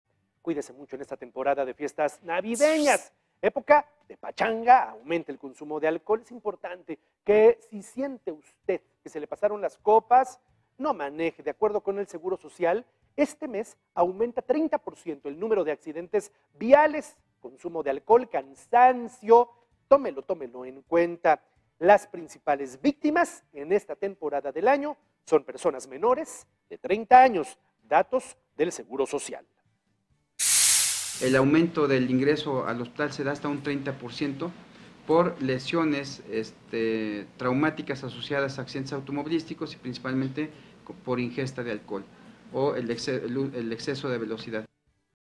Aumento de accidentes || Noticias con Juan Carlos Valerio
¡Mucho cuidado! En estas fiestas navideñas evite conducir si siente que se le pasaron las copas. En este mes aumenta un 30 % el número de accidentes viales.